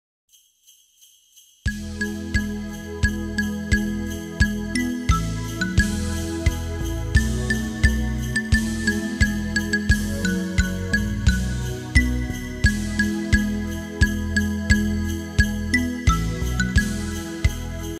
Les illuminations de Noël à Orange (Vaucluse) 2012
Le cours Aristide Briand
Mardi 4 décembre 2012
Brigitte CORDONNIER